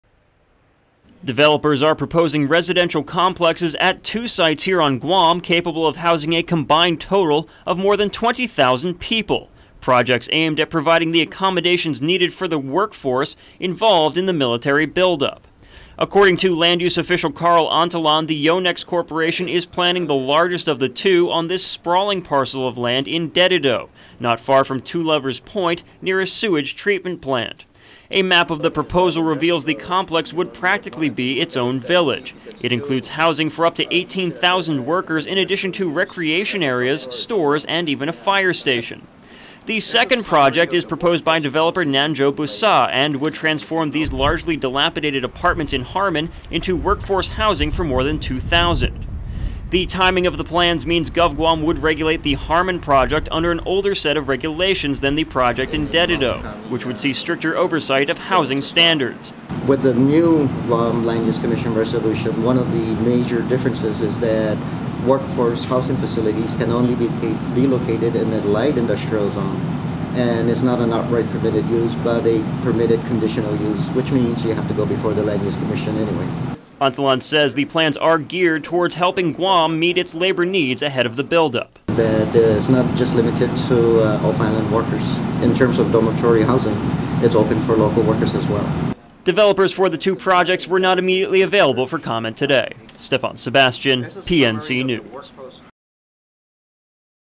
Ahead of Mil. Buildup, Worker Housing Plans Begin
Guam-- Developers are planning for the influx of new workers on Guam the pending military buildup will require.